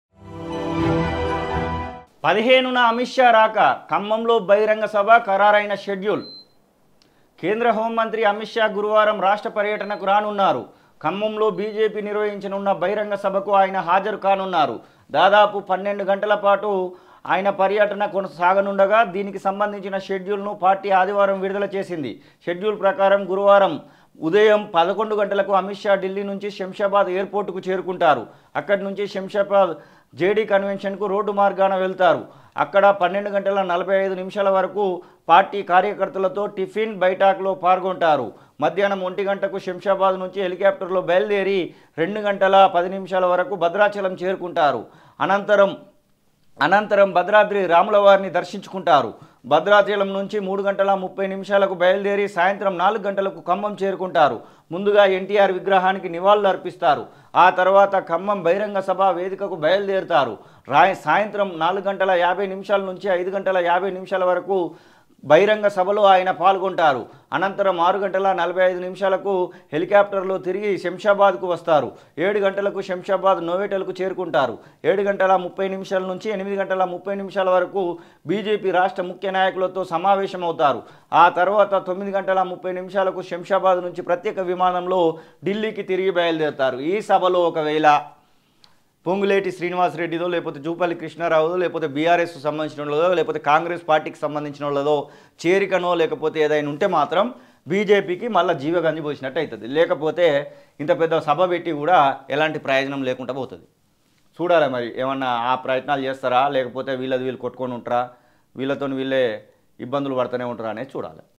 15 న అమిత్ షా సభ...! లుకలుకలకు ఫుల్ స్టాప్ పడేనా...? | Amith Shah Khammam Meeting | Tolivelugu TV
15 న అమిత్ షా సభ...! లుకలుకలకు ఫుల్ స్టాప్ పడేనా...? | Amith Shah Khammam Meeting | Tolivelugu TV